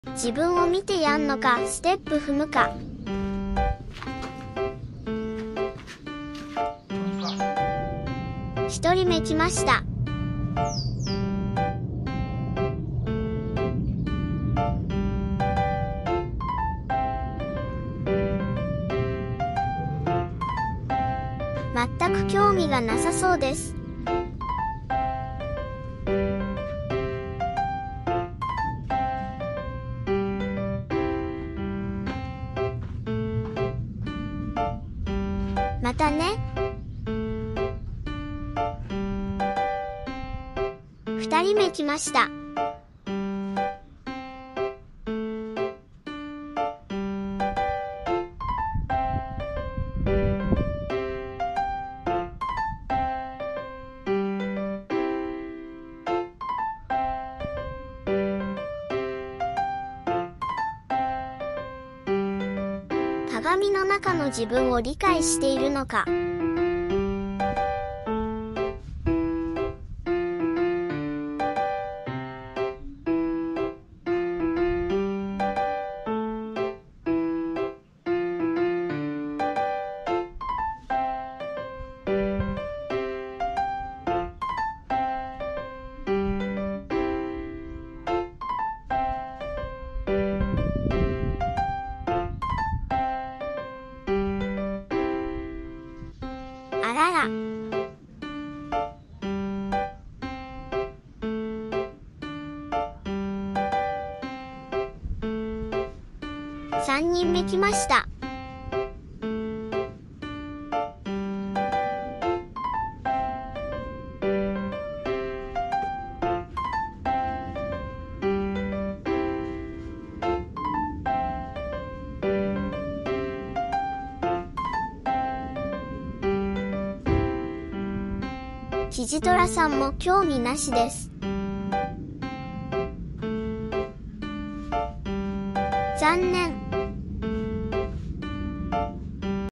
やんのかステップ踏む？
鏡の自分を見て猫はやんのかステップ踏むのか